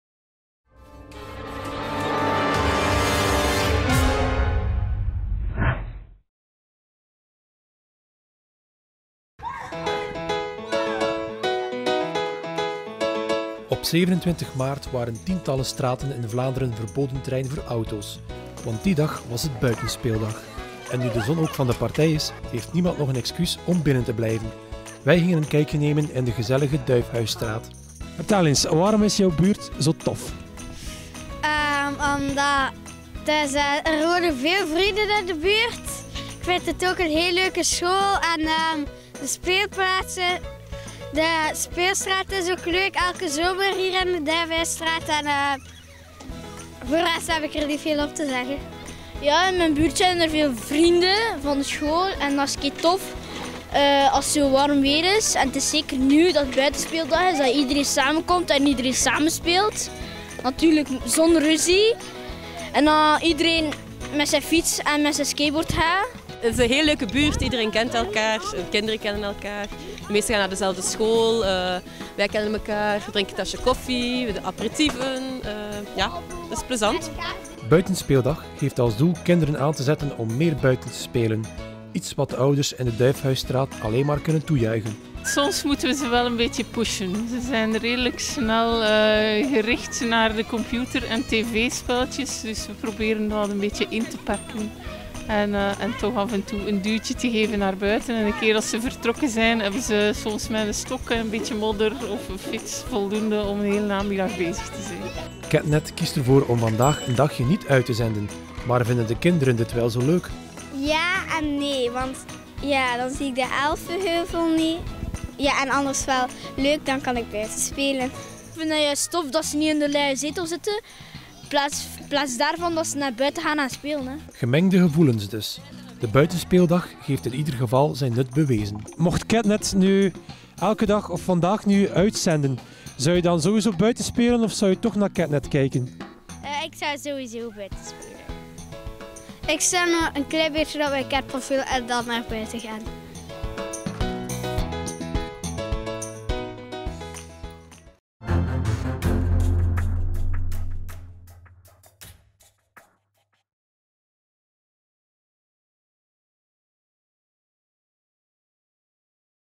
Opgelet! Hier speelt men...
De zon hoog aan hemel en het geluid van spelende kinderen op straat. De jaarlijkse Buitenspeeldag was opnieuw een succes! Ook bij Ketnet en Nickelodeon kreeg men de smaak te pakken en ging iedereen buiten spelen. Voor een dagje werden de stekkers van de zenders uitgetrokken!